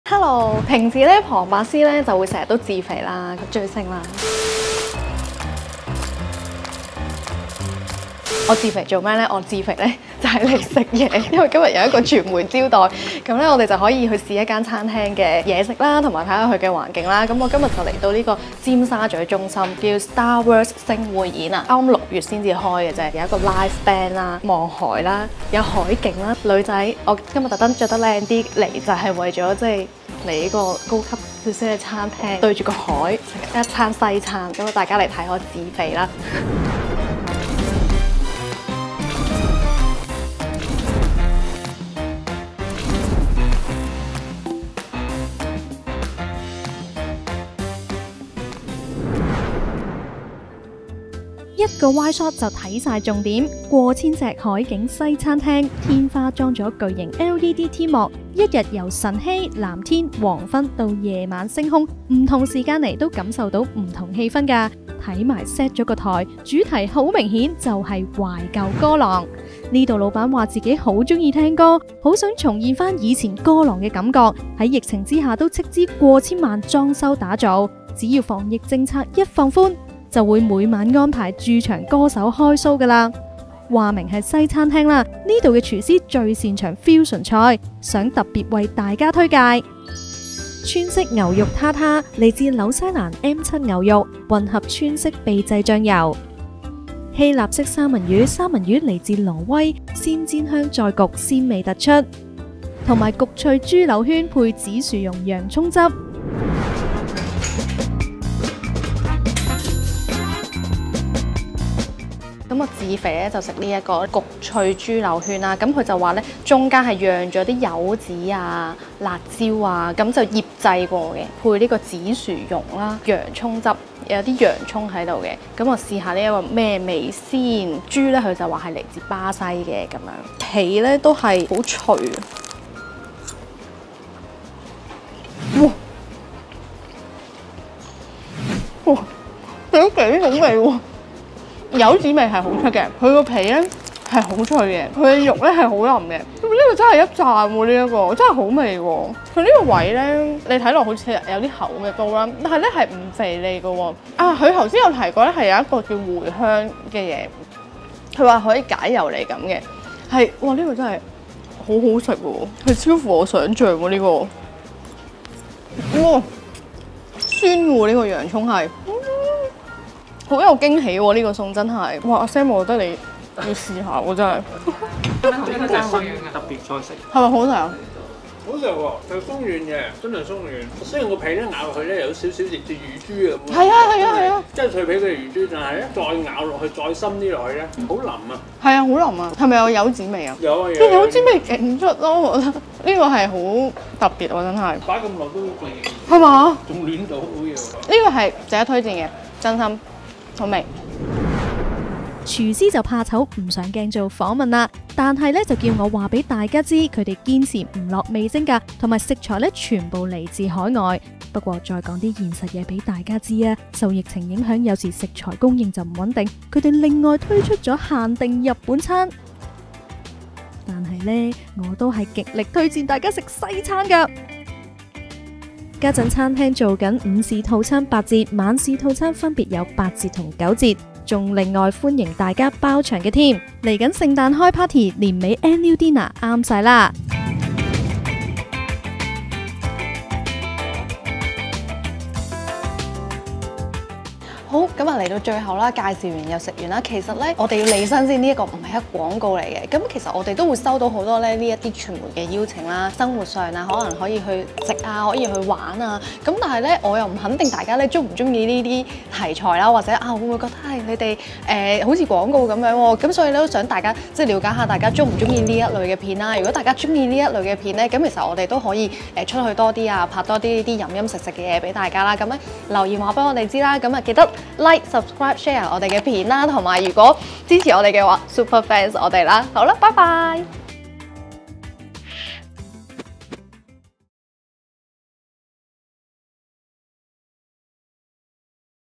阿仙自肥｜望住180度海景嘆Fusion菜 焗脆豬柳圈有驚喜抵讚 盼放寬防疫重現懷舊歌廊｜Channel C HK
利申，非廣告！
平時旁白師自肥得多，今次輪到阿仙自肥帶大家去食好西！Channel C較少做一些生活吃喝玩樂推薦，今次阿仙就試一試應邀去一個傳媒招待，到尖沙咀中心一間西餐廳StarVerse星匯演做試食體驗。詳情：

StarVerse 星匯演
地址：尖沙咀麼地道66號尖沙咀中心3樓
電話：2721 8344 / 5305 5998 (WhatsApp)
星期一至日：12:00 - 23:59